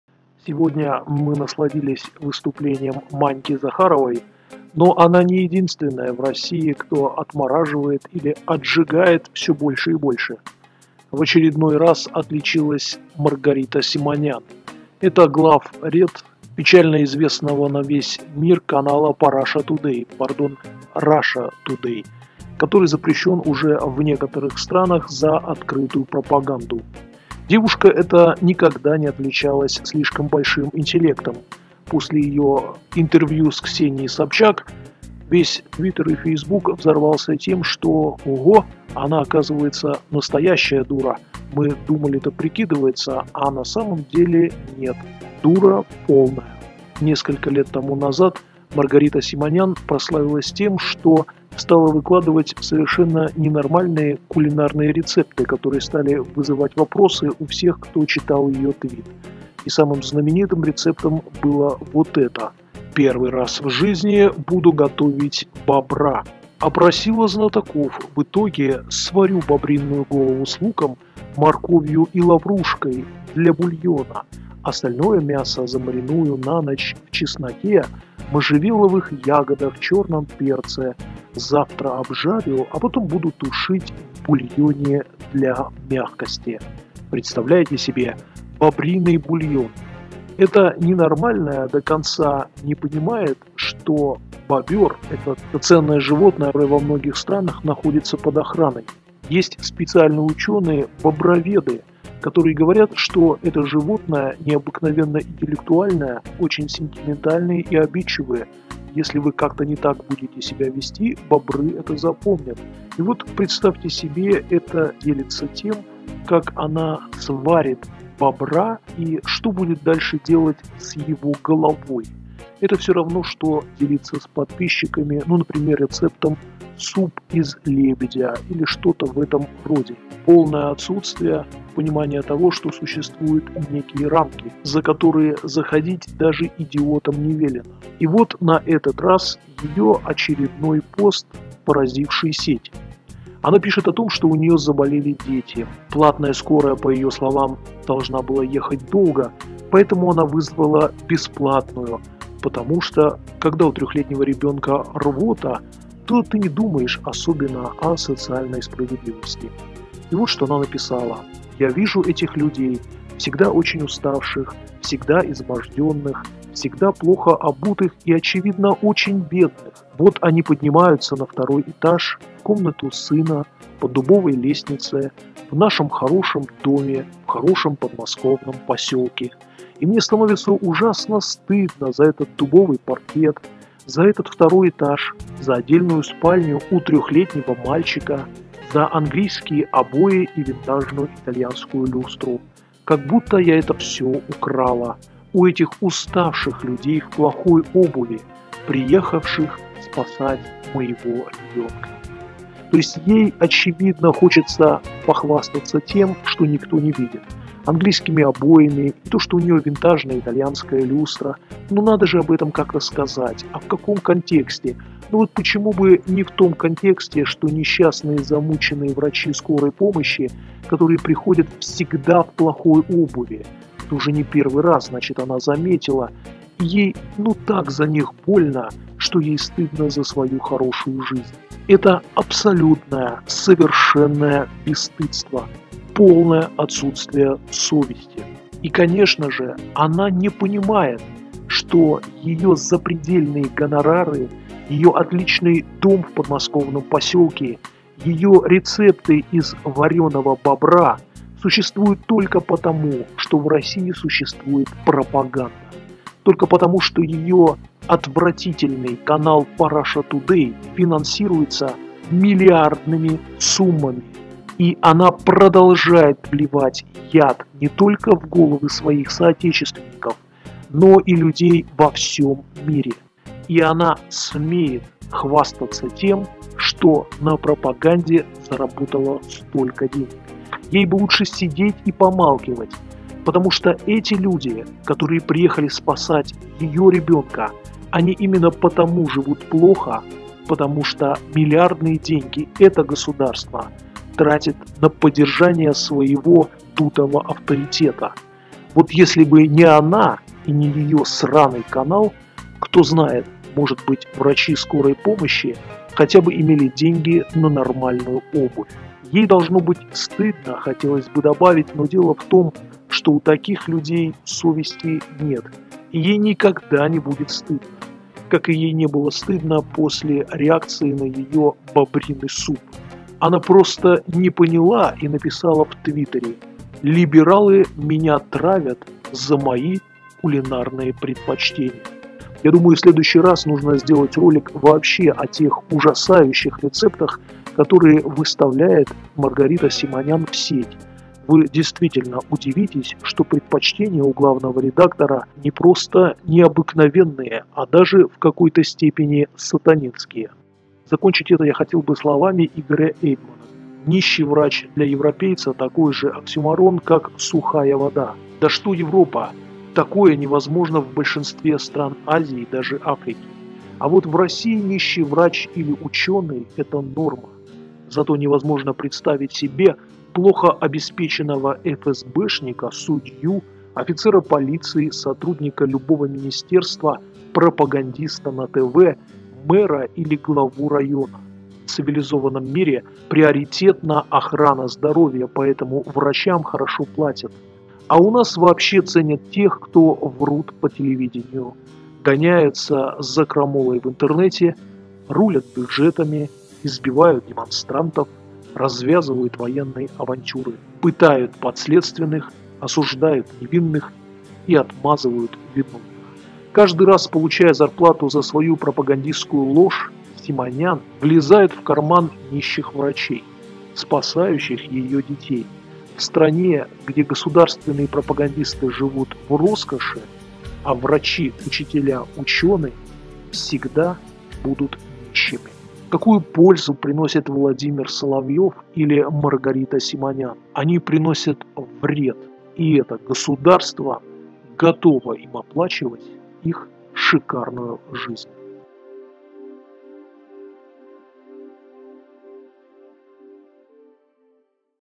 ⛑Маргарита Симонян и бедные врачи
Главный редактор Раша Тудей умеет не только варить бобров, но и "поболеть" за нищих  врачей.